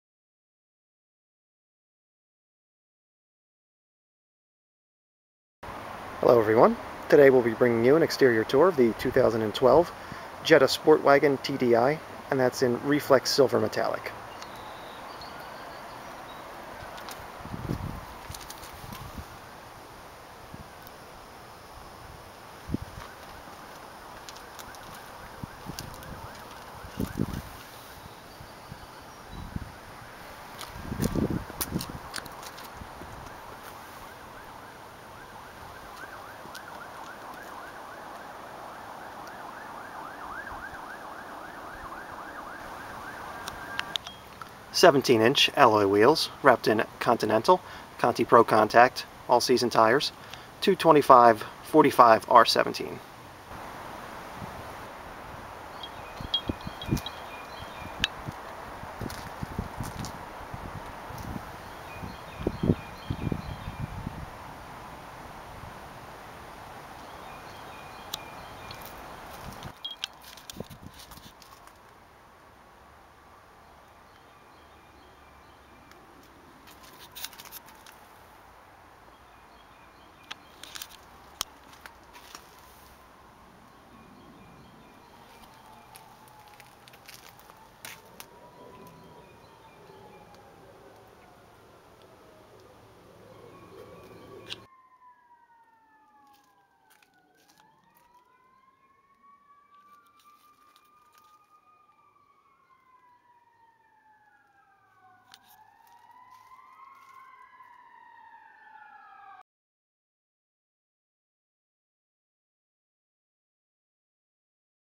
2012 Volkswagen Jetta SportWagen TDI
Walk around and tour of the 2012 Volkswagen Jetta SportWagen TDI in Reflex Silver Metallic.